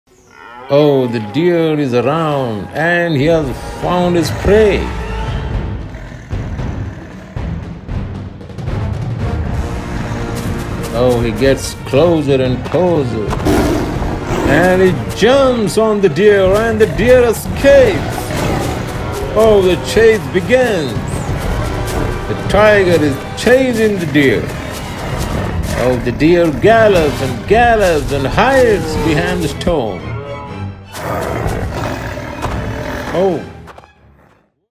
Tiger VS Deer | Wild Animals chasing | Wild Animals Fights compilation 2023
Watch Tiger VS Deer | Wild Animals chasing | Wild Animals Fights compilation 2023.